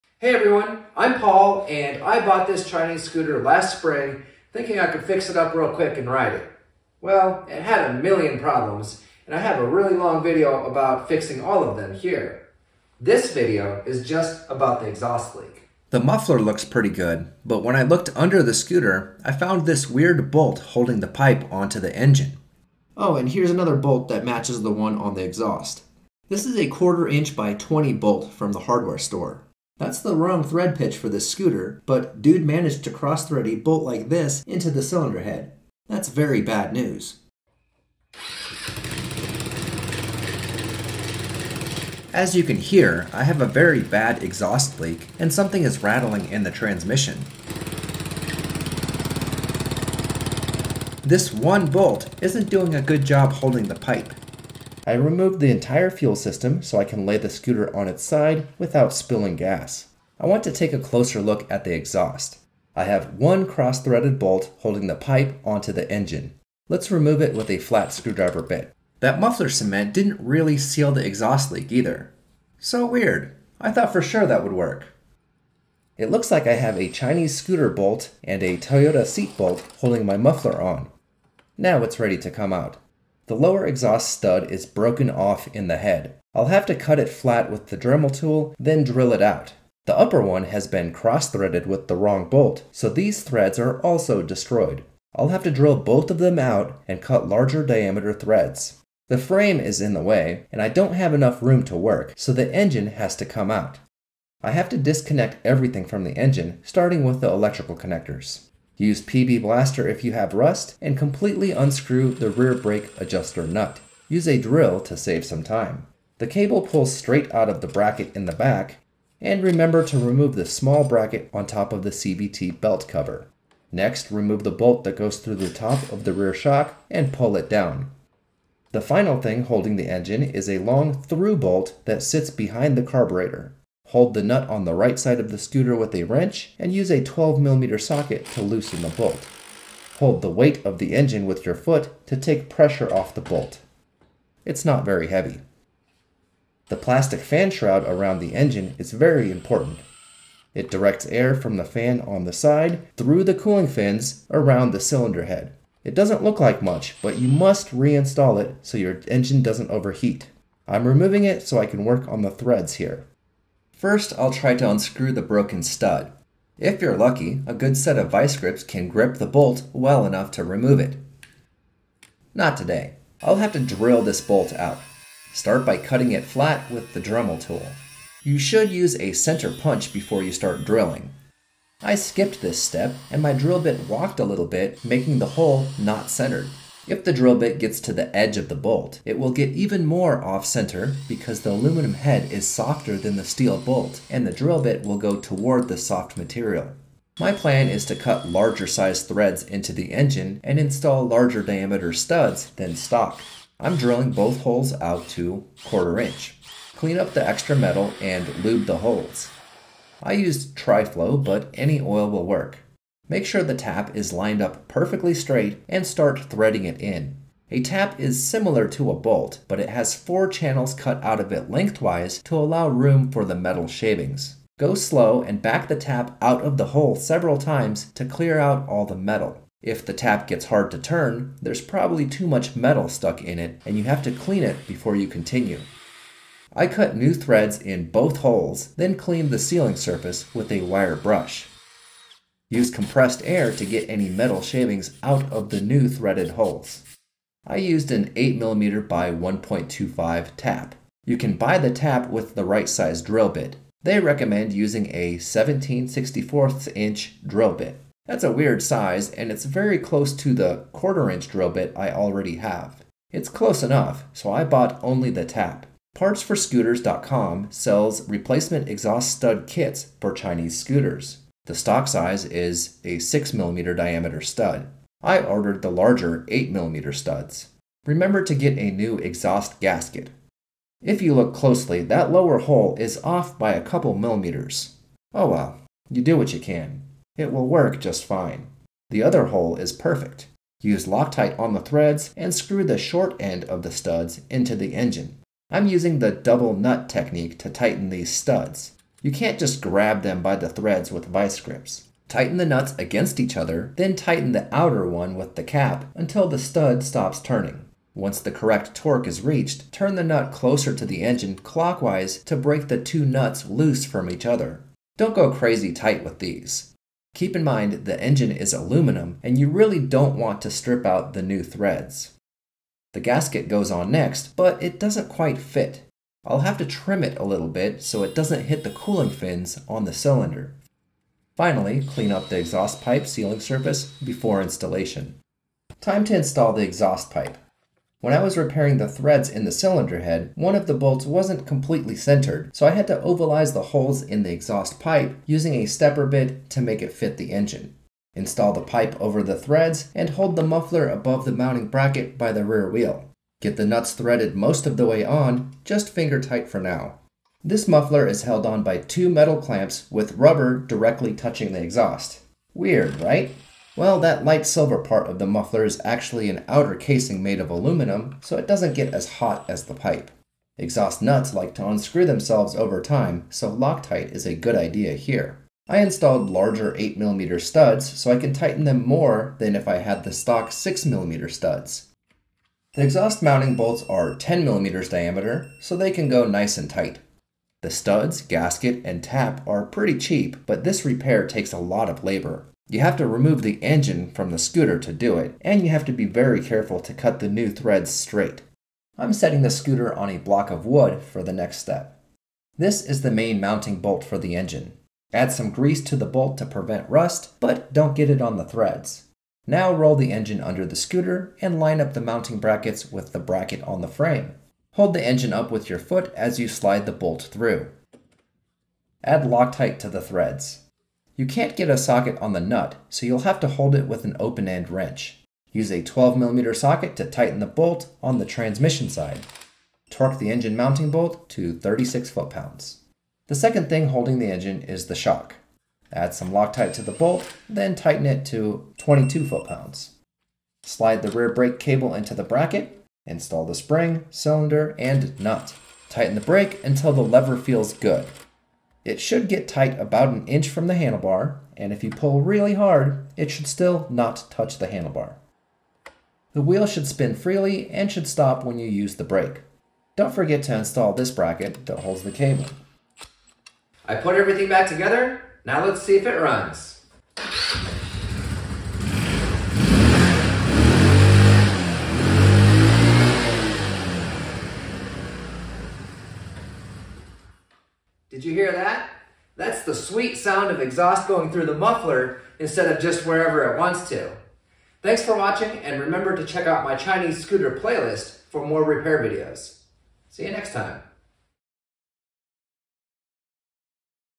Replacing exhaust stud and tapping new threads into cylinder head - 150cc GY6 Chinese scooter
Torque specifications:
Exhaust studs to head (8mm size):  7.3 ft-lb or 10 Nm
Exhaust bracket to engine bolts:  18 ft-lb or 24 Nm
Exhaust nut torque (6mm studs):  8 ft-lb or 11 Nm
Exhaust nut torque (8mm studs):  12 ft-lb or 16 Nm
Muffler clamp bolts:  30 ft-lb or 40 Nm
Engine mounting bolt:  36 ft-lb or 49 Nm
Shock bolt to frame:  22 ft-lb or 30 Nm

Parts:
Exhaust studs 6mm (stock) or 8mm

Tools:
8mm x 1.5 tap

0:00 intro
1:02 remove the muffler
1:44 remove the engine
2:26 remove the fan shroud
2:43 drill out old studs from cylinder head
3:29 tap new threads
4:40 install new exhaust studs
5:22 install muffler
6:38 install engine
7:20 connect rear brake
7:43 conclusion